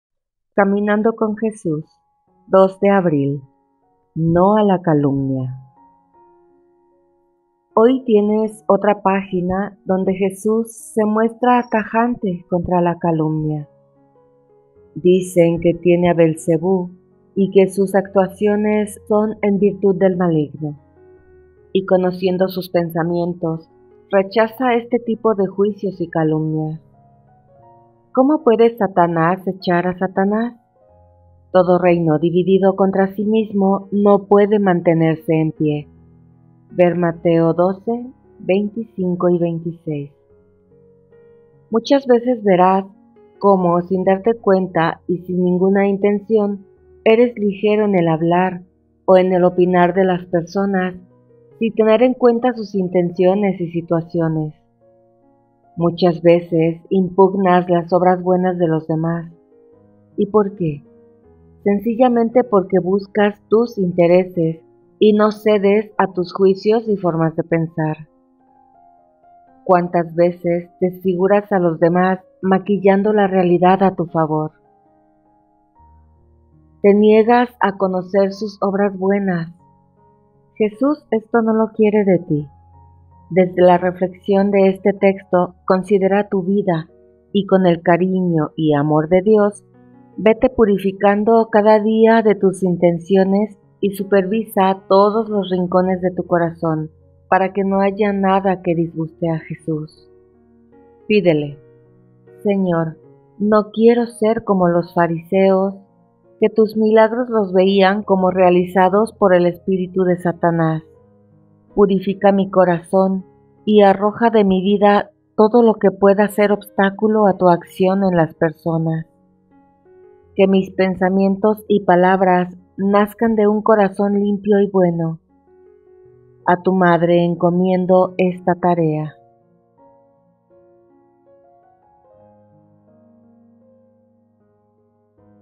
Caminando con Jesús 02 de abril "No a la calumnia"
Meditaciones de Francisca Sierra

Caminando con Jesús

02 de abril
2 de abril
No a la calumnia
Hoy tienes otra página donde Jesús se muestra tajante contra la calumnia. Dicen que tiene a Belcebú y que sus actuaciones son en virtud del Maligno. Y conociendo sus pensamientos, rechaza este tipo de juicios y calumnias: «¿Cómo puede Satanás echar a Satanás? Todo reino dividido contra sí mismo no puede mantenerse en pie» (cf Mt 12,25-26).
Muchas veces verás cómo sin darte cuenta y sin ninguna intención eres ligero en el hablar y en el opinar de personas, sin tener en cuenta sus intenciones y situaciones.
Muchas veces impugnas las obras buenas de los demás. Y, ¿por qué? Sencillamente porque buscas tus intereses y no cedes en tus juicios y formas de pensar.

¡Cuántas veces desfiguras a los demás maquillando la realidad a tu favor! Te niegas a conocer sus obras buenas. Jesús esto no lo quiere en ti. Desde la reflexión de este texto considera tu vida, y con el cariño y amor de Dios, vete purificando cada una de tus intenciones y supervisa todos los rincones de tu corazón para que no haya nada que disguste a Jesús.
Pídele: Señor, no quiero ser como los fariseos, que tus milagros los veían como
realizados por el espíritu de Satanás. Purifica mi corazón y arroja de mi vida todo lo
que pueda ser obstáculo a tu acción en las personas. Que mis pensamientos y palabras
nazcan de un corazón limpio y bueno. A tu Madre encomiendo esta tarea.